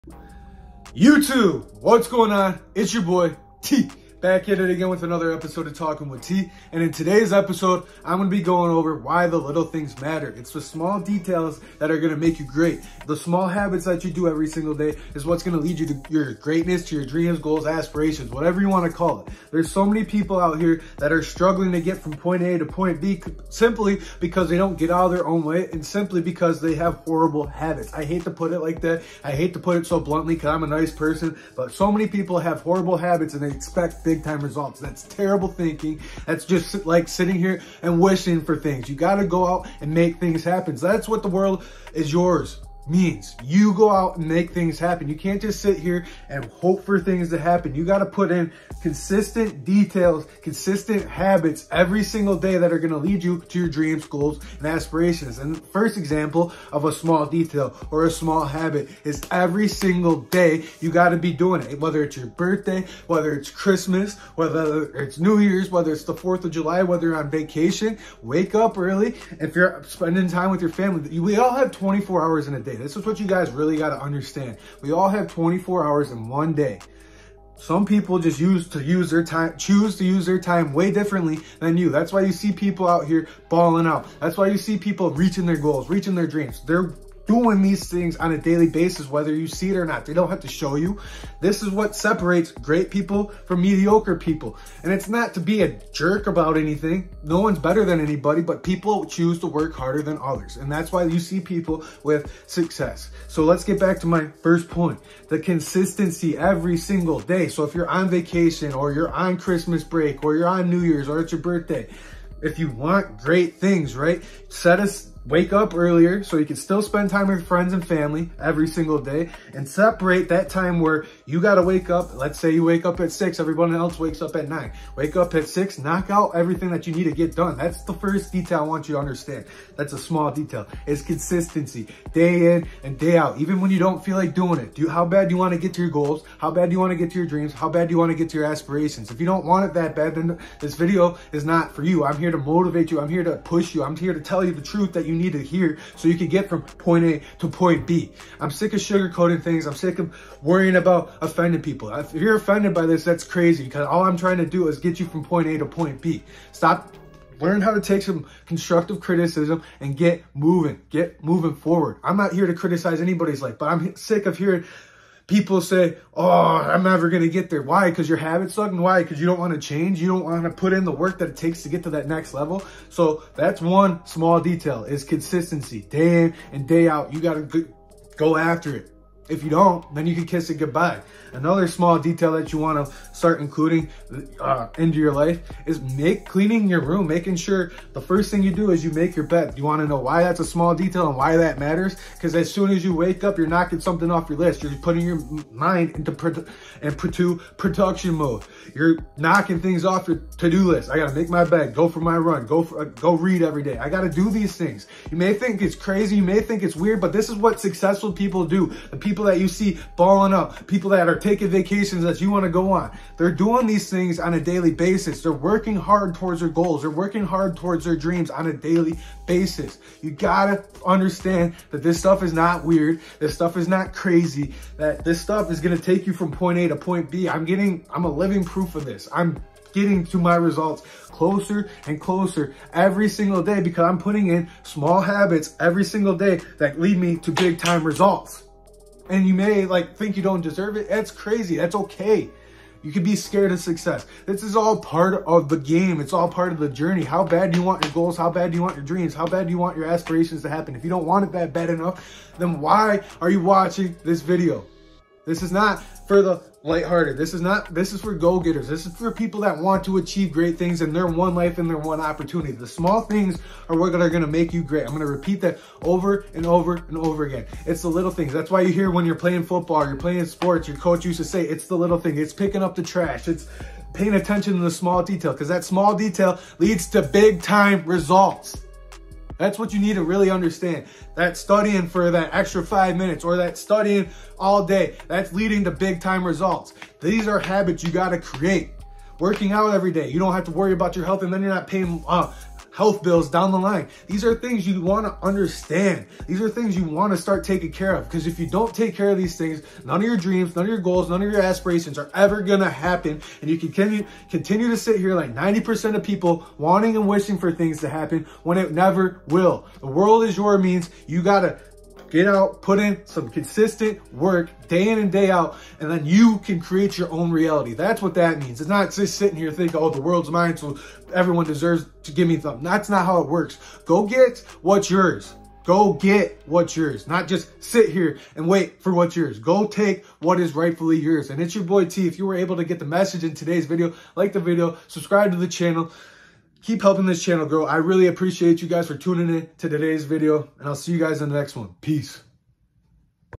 🚨MOTIVATION: THE SMALL HABITS ARE WHAT MAKE YOU GREAT🚨
• In todays video I talk about how important the small details and habits are you your success